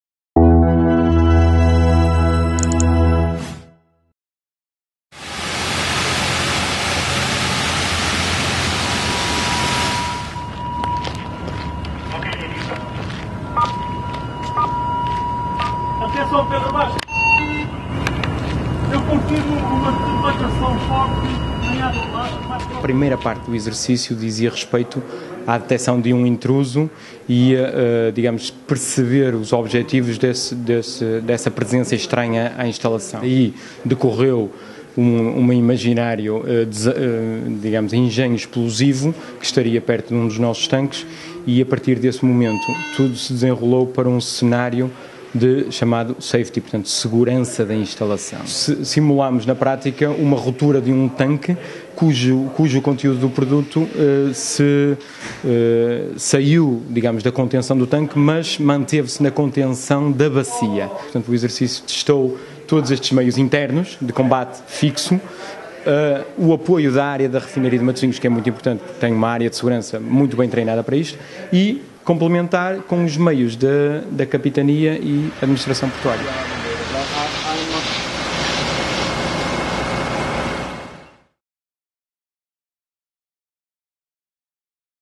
E se um tanque da Galp explodisse?
Um simulacro no Terminal Petroleiro do Porto de Leixões testou, esta terça-feira, a capacidade de resposta dos serviços de emergência em caso de acidente e rutura das infraestruturas.